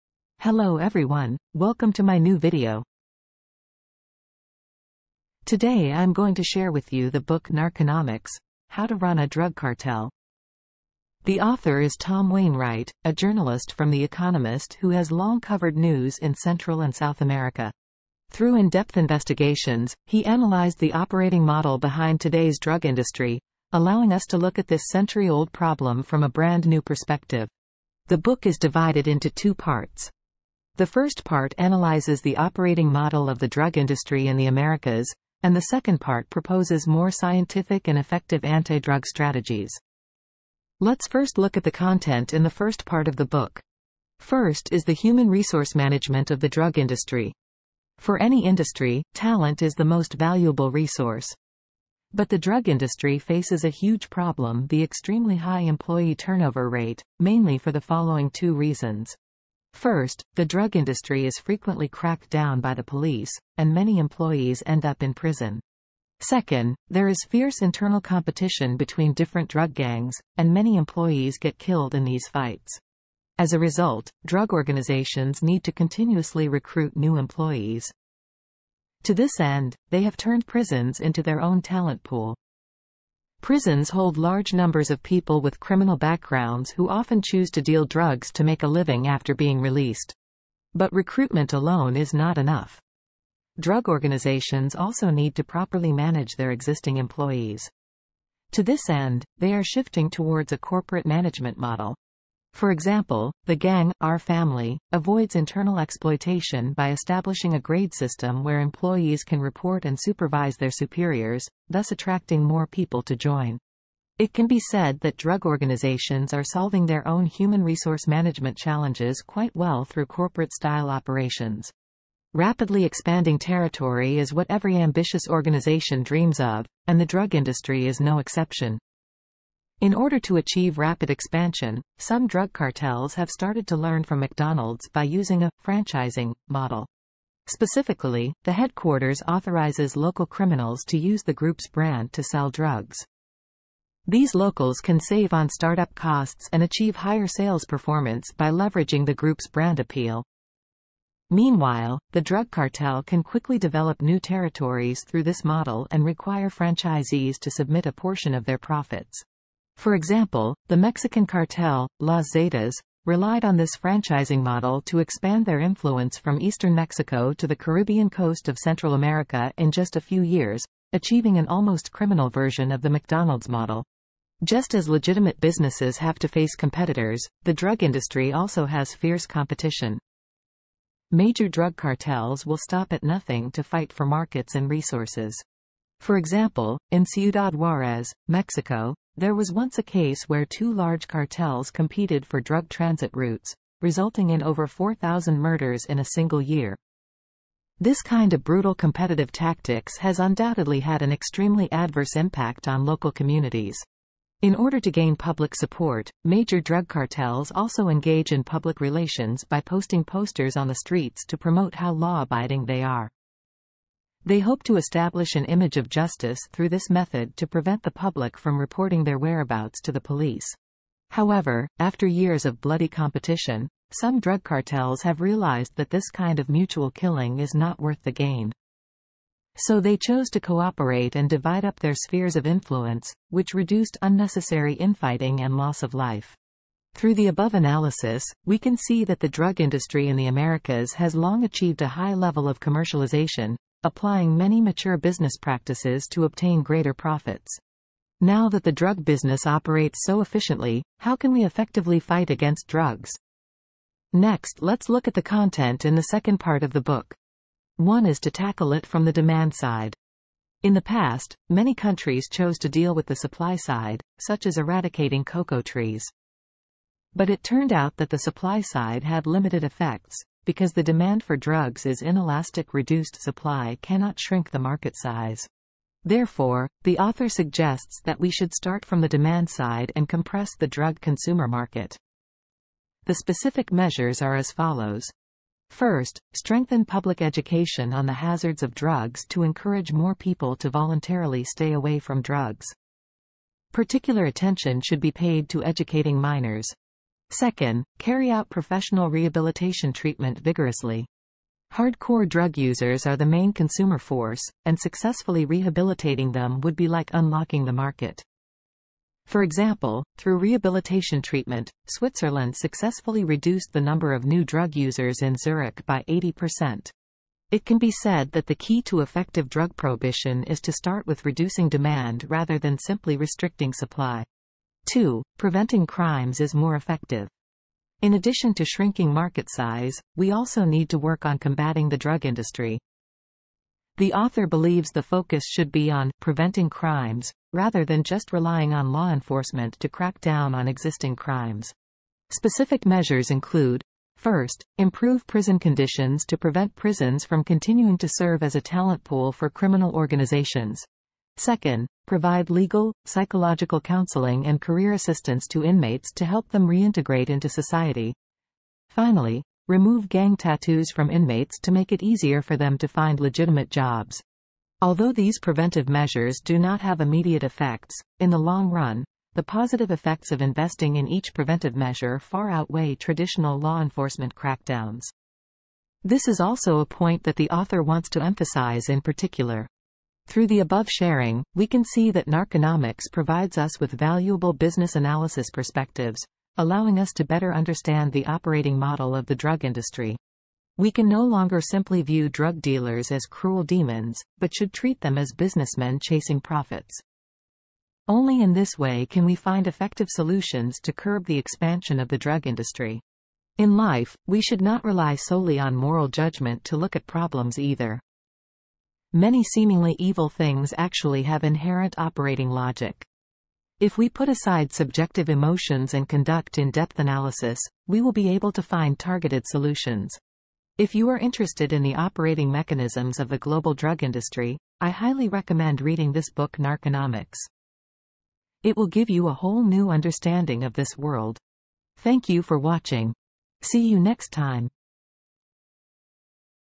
The Shocking Business Truth Behind Drug Cartels
Narconomics: The Shocking Business Truth Behind Drug Cartels 😱

Hey friends! 👋 Are you interested in learning the inside secrets behind how drug cartels operate their business? 🤔

In this video, I'm going to give you a complete overview of the book Narconomics: How to Run a Drug Cartel by Tom Wainwright. Through his in-depth investigation as a journalist at The Economist, Tom revealed the shocking business truth behind the world's most notorious drug cartels like you've never seen before! 😲

Here's a sneak peek of what you'll discover in this video:

👉 How cartels manage their "human resources" and recruitment 💼

👉 The franchising model cartels use to expand rapidly 🏢

👉 The economics behind cartels' competition and collaboration 🤝

👉 Why attacking supply doesn't reduce drug demand 📉

👉 How cartels market themselves through PR and branding 📢

👉 The key to running a drug business like a real corporation 💰

If you find topics like economics, business, cartels, and the drug trade interesting, you'll LOVE this video and what you'll learn from Narconomics! 📚

So get ready to have your mind blown about how professional and business-minded drug cartels have become! 🤯

I highly recommend you get the book Narconomics by Tom Wainwright to learn the full business case studies behind illegal drug cartels across the Americas and beyond. It will completely change how you see the economics of the drug trade!